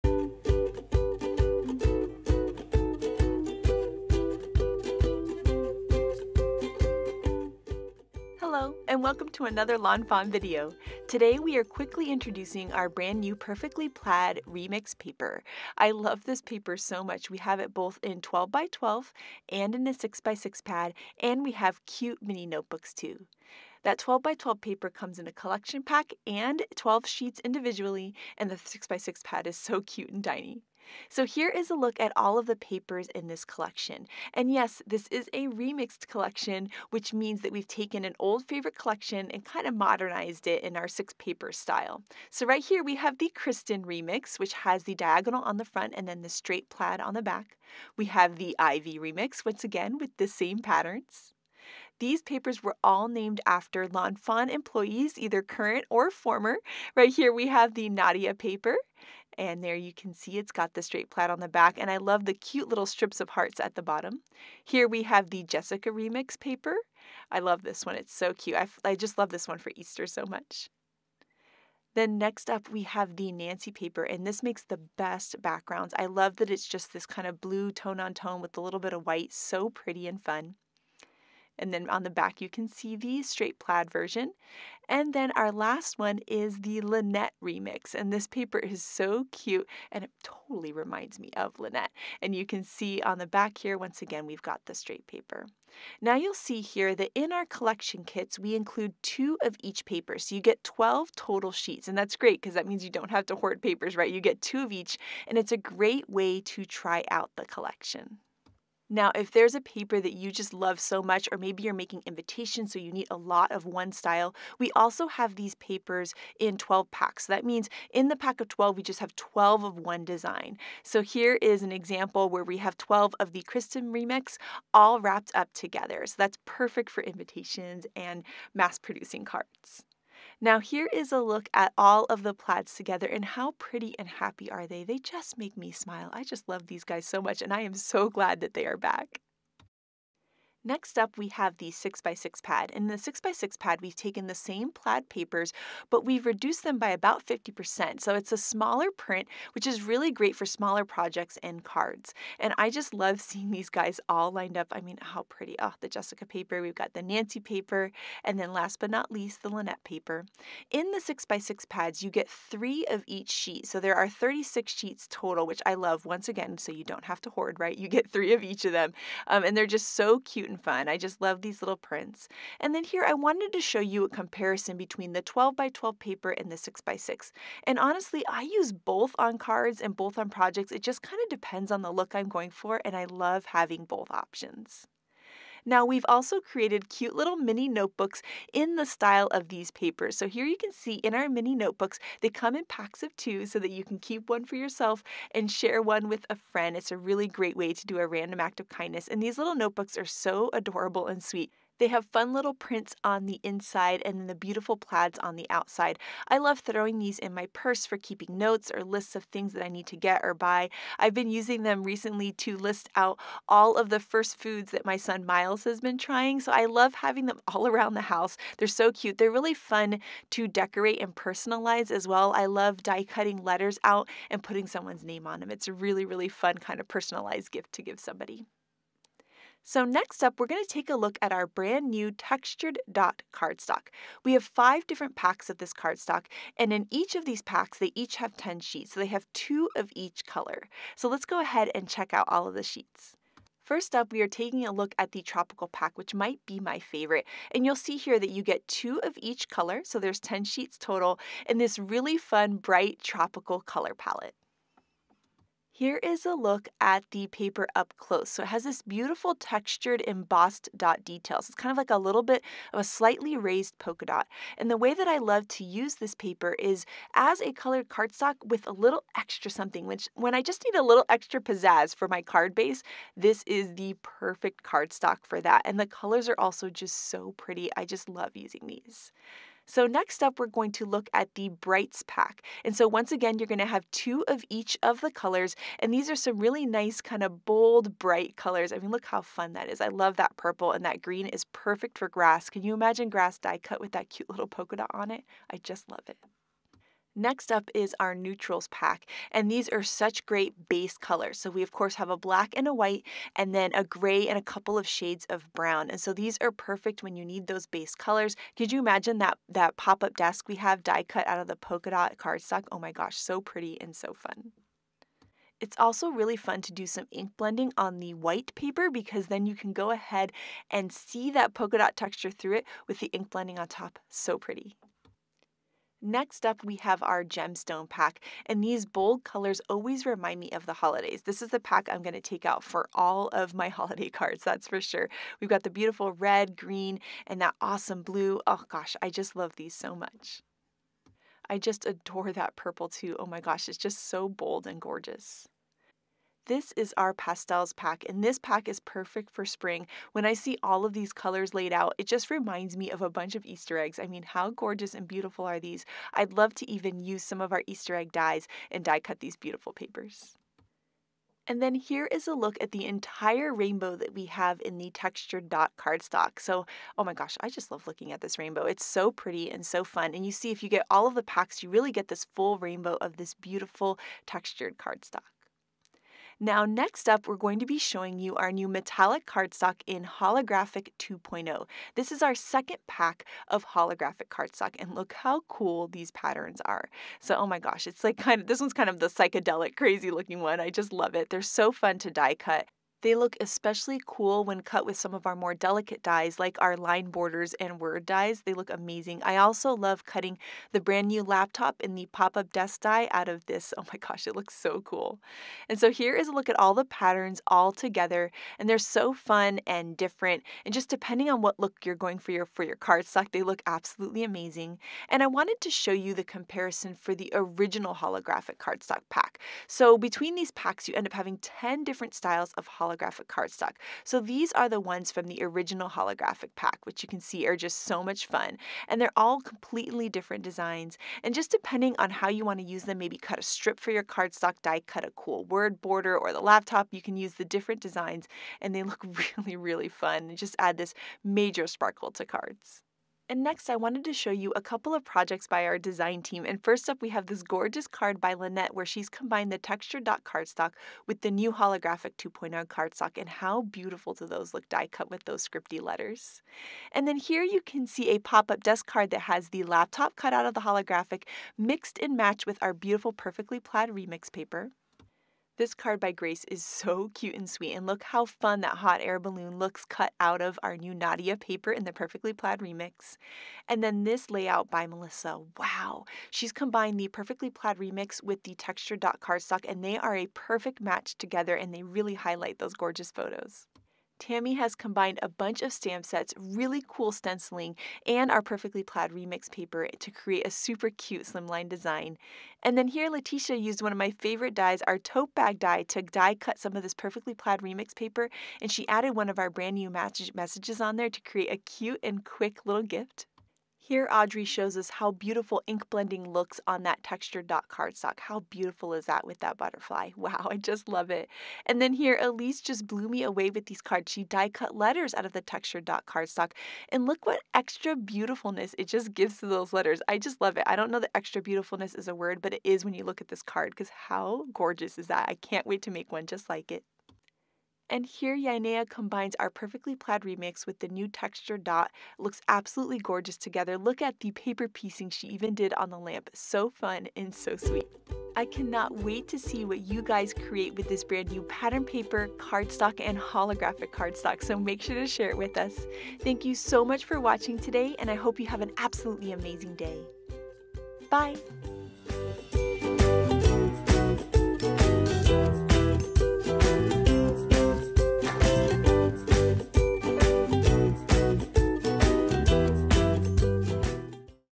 Intro to Perfectly Plaid Remix, Textured Cardstock + Holographic 2.0 Cardstock
to see our products, more ideas and inspiration! In this quick video, Lawn Fawn owner Kelly Alvarez introduces our new paper collection Perfectly Plaid Remix and new cardstock Textured Dot and Holographic 2.0